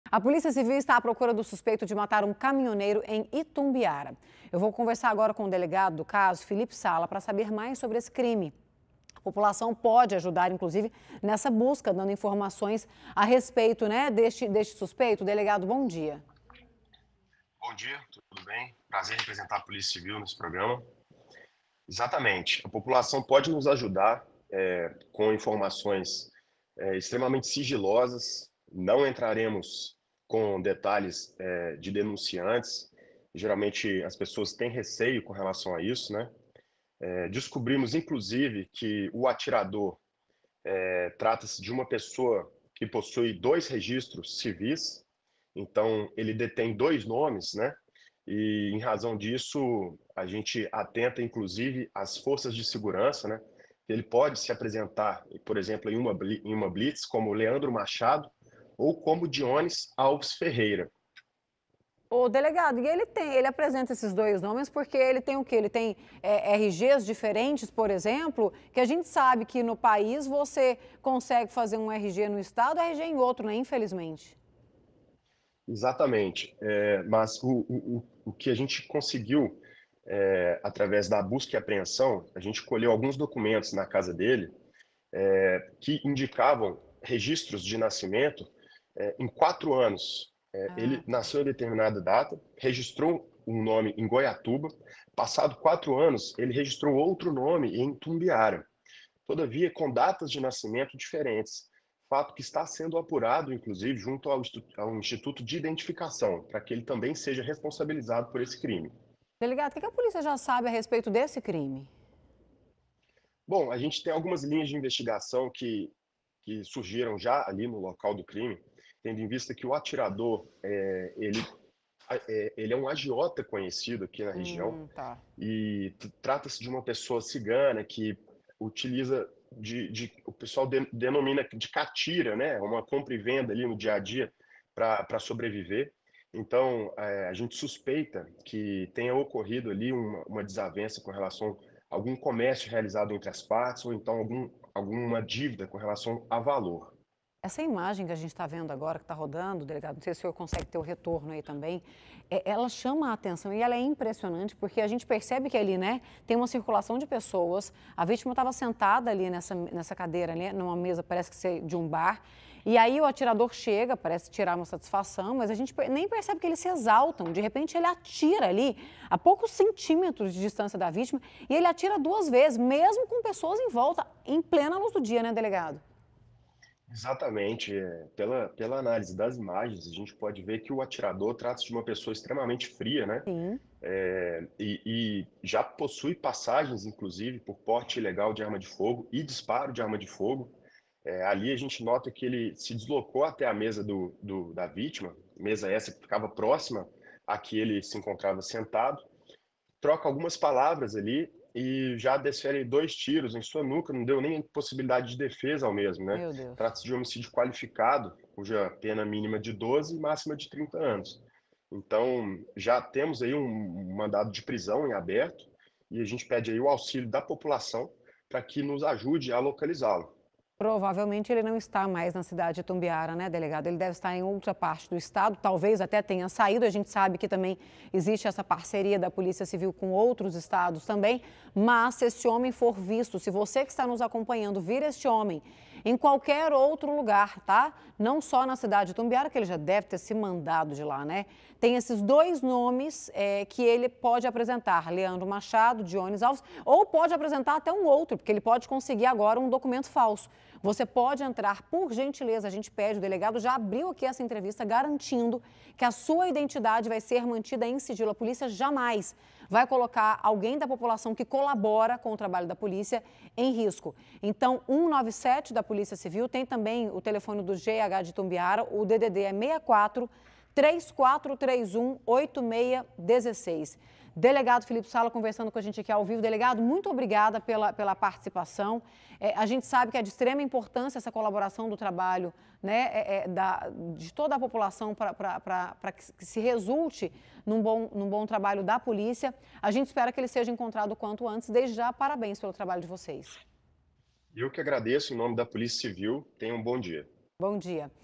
CRIME A LUZ DO DIA: HOMEM É PROCURADO POR HOMICÍDIO EM ITUMBIARA
A POLÍCIA CIVIL ESTÁ A PROCURA DO SUSPEITO DE MATAR UM CAMINHONEIRO, EM ITUMBIARA.